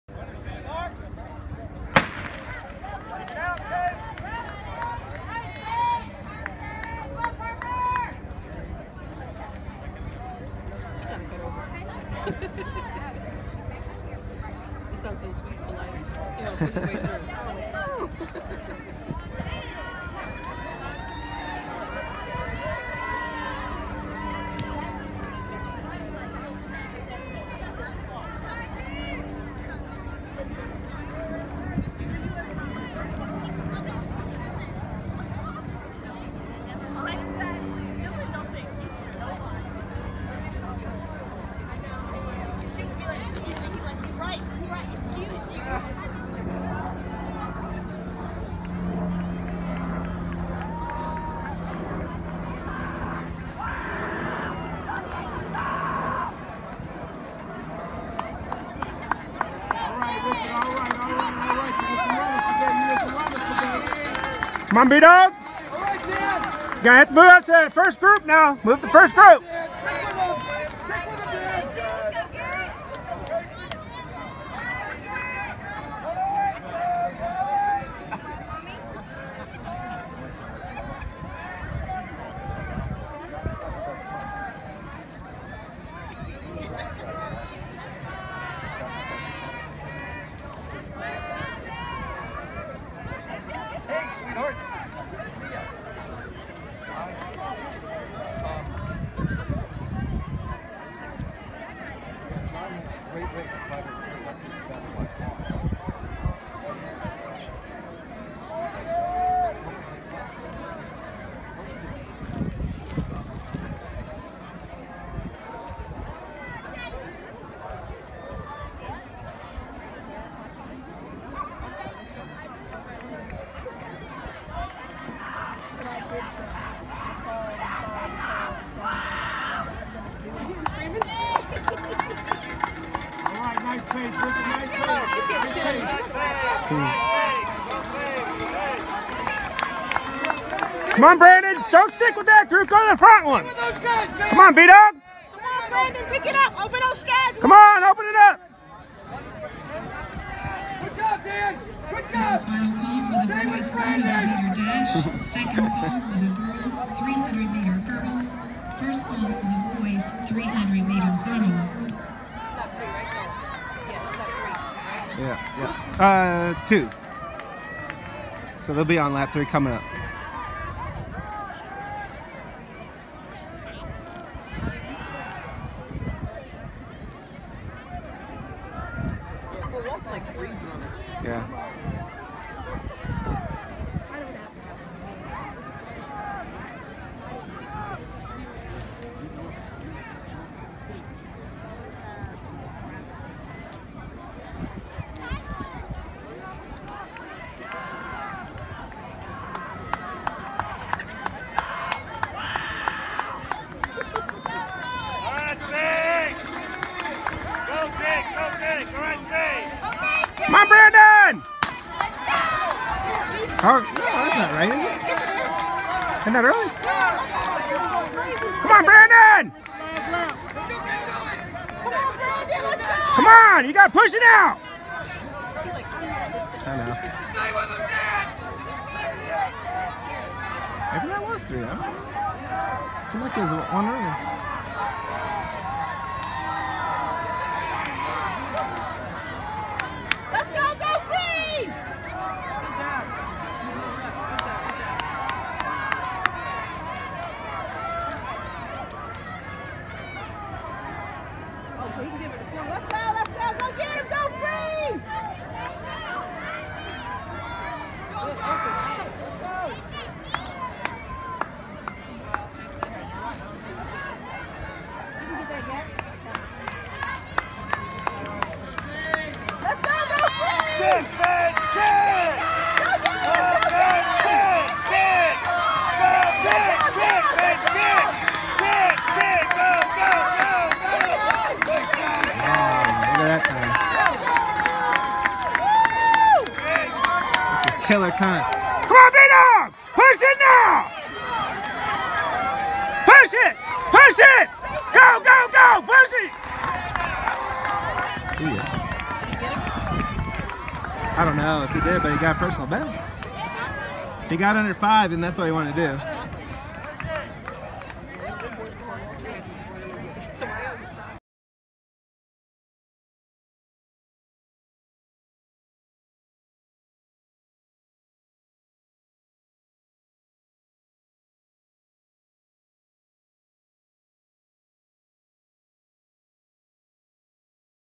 2018 Frank Lay Patriot Invitational - 1600 Meter Race(heat 2) - 3/2/2018
Brandon Knieja running 1600 Meter Race with a PR time of 4:56.43 for Tate High School